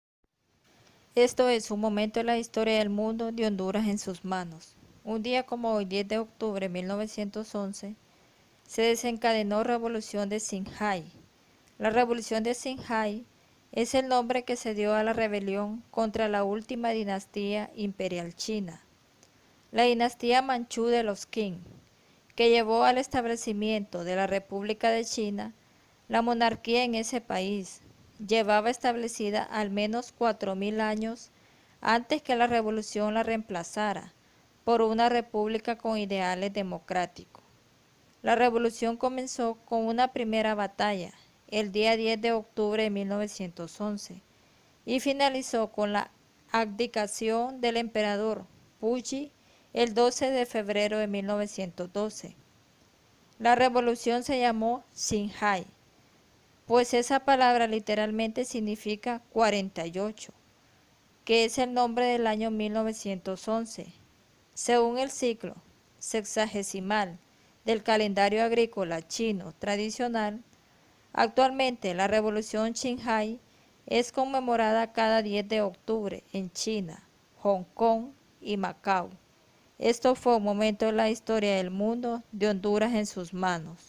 10 de octubre 1911, Se desencadeno Revolucion de Xinhai
La Revolución de Xinhai es el nombre que se dio a la rebelión contra la última dinastía imperial china, la dinastía manchú de los Qing, que llevó al establecimiento de la República de China. La monarquía en ese país llevaba establecida al menos 4000 años antes que la Revolución la reemplazara por una república con ideales democráticos. La revolución comenzó con una primera batalla el día 10 de octubre de 1911 y finalizó con la abdicación del Emperador Puyi el 12 de febrero de 1912. La revolución se llamó Xinhài pues esa palabra literalmente significa "cuarenta y ocho", que es el nombre del año 1911, según el ciclo sexagesimal del calendario agrícola chino tradicional. Actualmente la Revolución Xinhai es conmemorada cada 10 de octubre en China, Hong Kong y Macau.